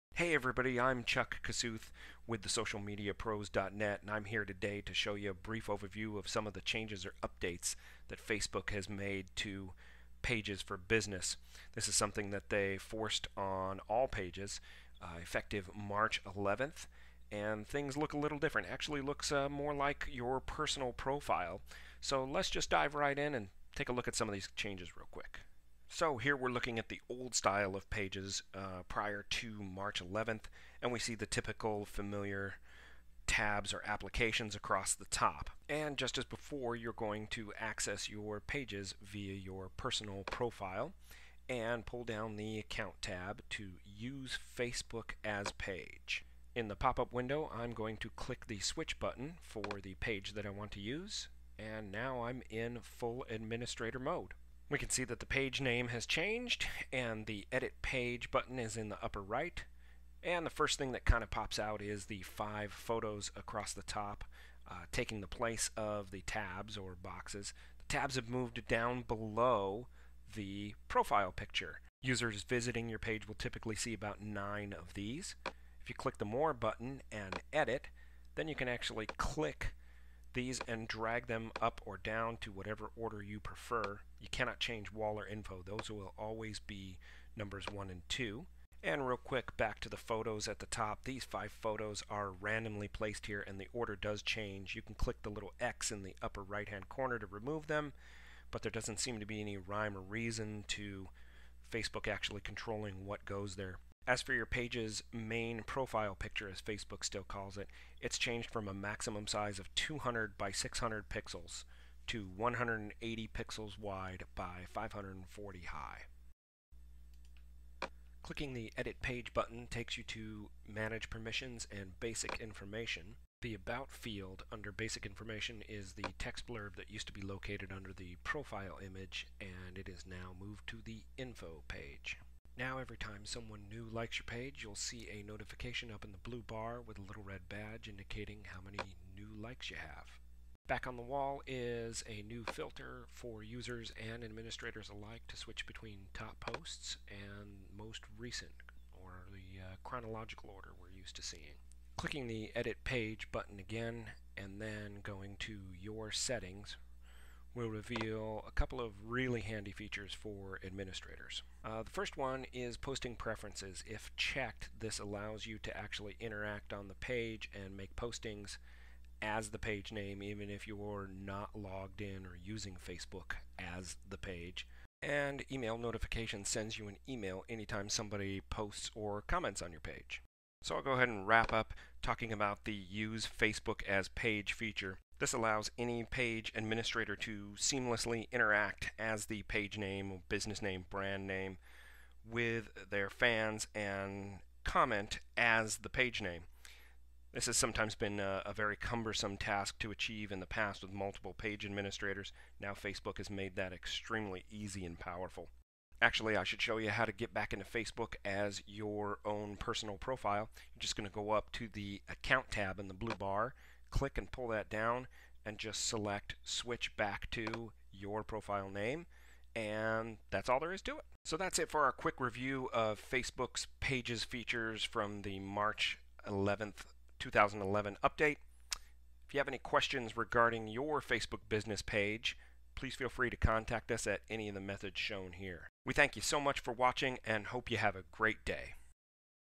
An Brief Overview of Facebook's Business Page Updates
Chuck Kossuth from theSocialMediaPros.net gives business owners a quick overview of the updates made to pages on March 11th, 2011